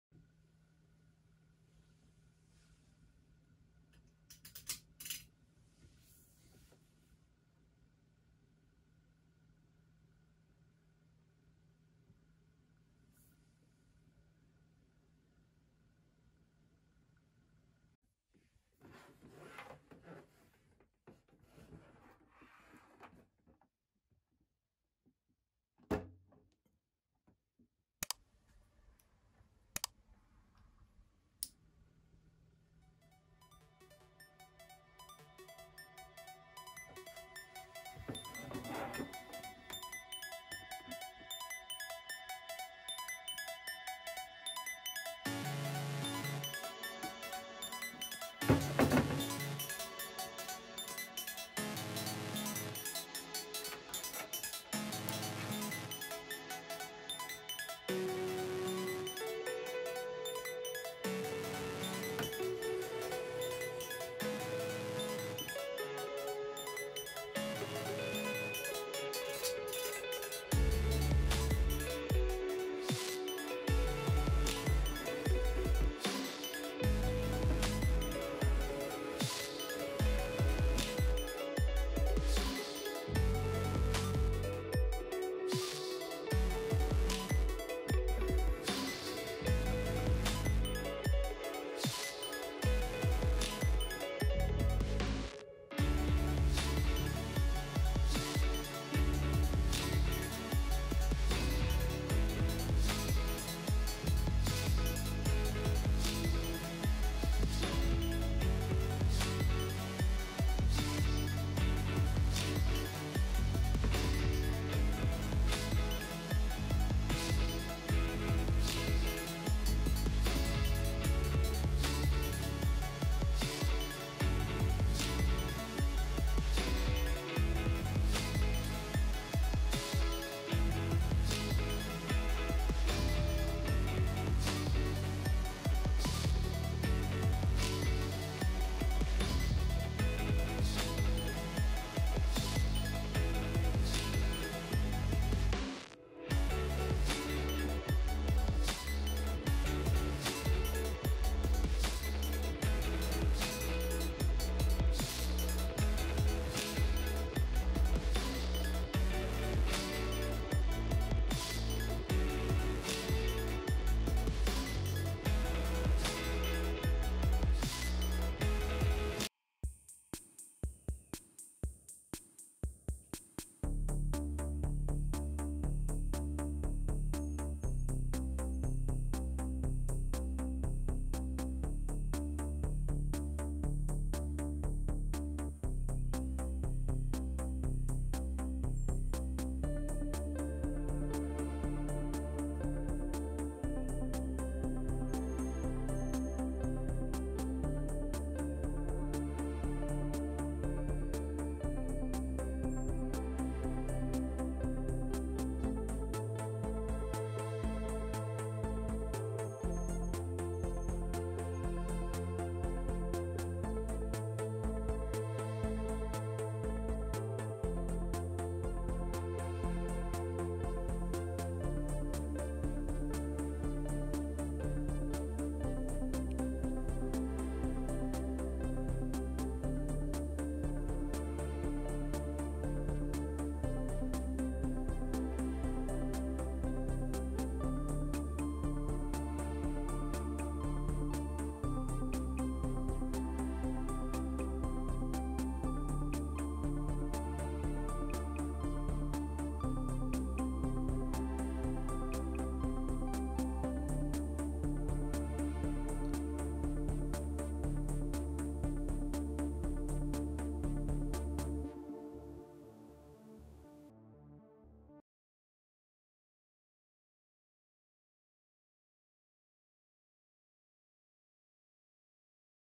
Color Changing Fiber Optic Peacock Lamp -- Gadgetify
Here is a cool fiber optic peacock lamp that puts a cool show at night. It changes color automatically when you turn it on. The peacock feathers change in waves to display beautiful rainbow colors.

This optical fiber peacock lamp is not easy to find but your best bet is eBay, Etsy or thrift shops.